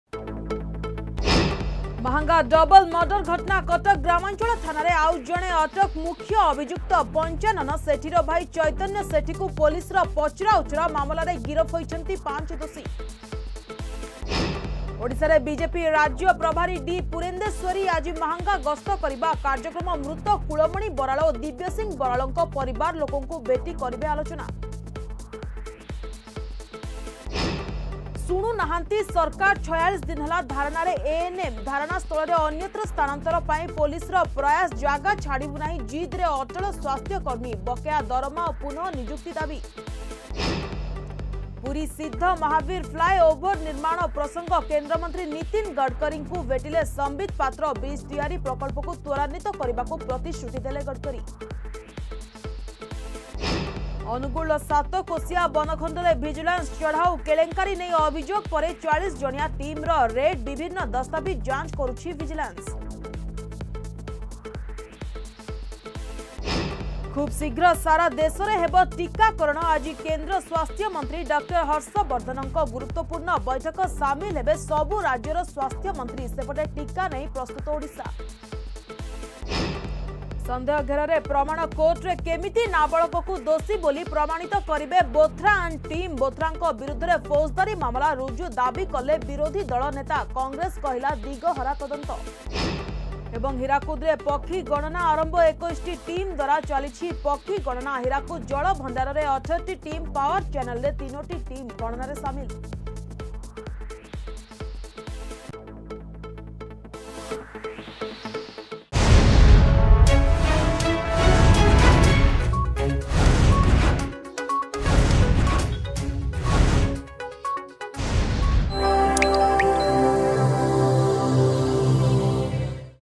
Headlines 10Am
ମାହାଙ୍ଗା ଡବଲ୍ ମର୍ଡର ଘଟଣା...
ଓଡ଼ିଶାରେ ବିଜେପି ରାଜ୍ୟ ପ୍ରଭାରୀ ଡି.ପୁରନ୍ଦେଶ୍ୱରୀ...
ଶୁଣୁନାହାଁନ୍ତି ସରକାର...
ପୁରୀ ସିଦ୍ଧ ମହାବୀର ଫ୍ଲାଏ ଓଭର ର୍ନିମାଣ ପ୍ରସଙ୍ଗ...
ଅନୁଗୁଳ ଓ ସାତକୋଶିଆ ବନଖଣ୍ଡରେ ଭିଜିଲାନ୍ସ ଚଢାଉ...
ଖୁବ ଶୀଘ୍ର ସାରା ଦେଶରେ ହେବ ଟୀକାକରଣ...
ସନ୍ଦେହ ଘେରରେ ପ୍ରମାଣ...
ହୀରାକୁଦରେ ପକ୍ଷୀ ଗଣନା ଆରମ୍ଭ...